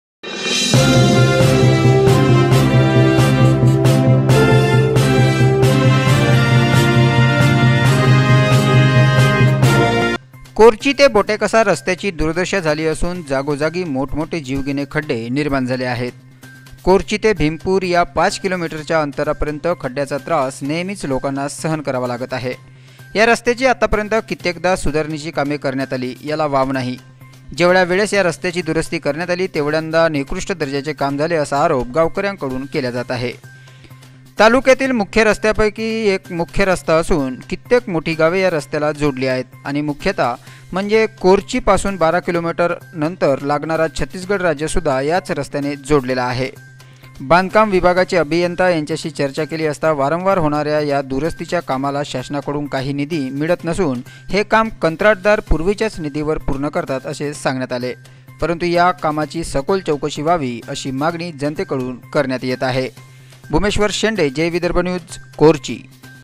कोरची-बोटेकसा मार्गाची दुुरवस्था
वारंवार होते निकृष्ठ दर्जाचे काम:नागरीकांची चैकशीची मागणी